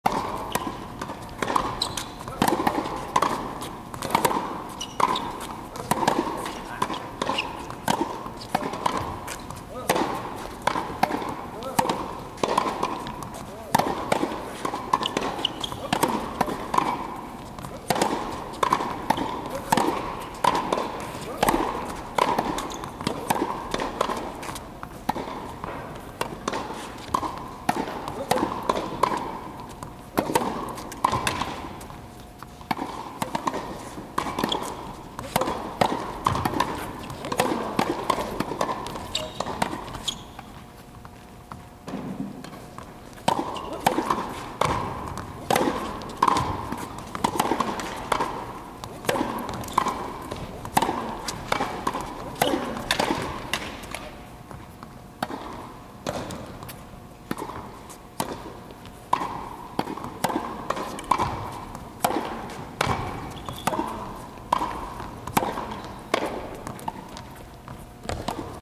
松井俊英(Toshihide Matsui)選手のボレー練習 上手いボール捌き
松井俊英選手のボレー練習。さすがにサーブ＆ボレーの名手、ボレーの球さばきははうまい。

松井俊英選手のボレー、テークバックが小さくフォロースルーでボールコントロールしてますね。同じくサーブ＆ボレーの名手、日本の鈴木貴男選手に比べるとボレーのキレは鈴木貴男選手の方がありますけどね。

体の側面より後ろにテークバックをしてないですね。きれいなボレー。